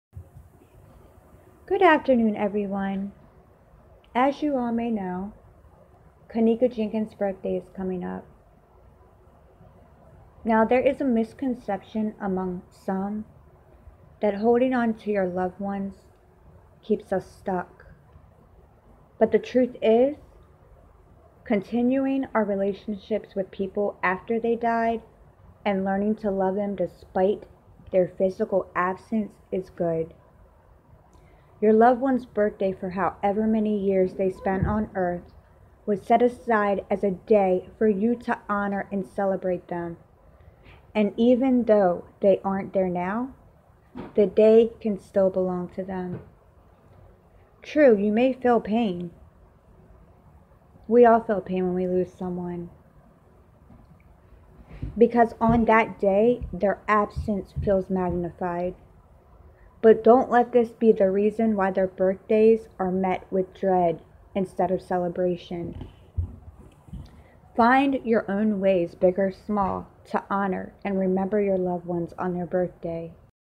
Kenneka's birthday celebration and honoring loved ones birthdays after they are no longer with us 💜
Kenneka's birthday celebration is going to be a celebration to remember the good times of Kenneka's short life. Despite being deceased does not mean that birthdays need to stop there's ways to remember a loved one and celebrate memories long after they leave this Earth. Justice for Kenneka Jenkins and J4all VICTIMS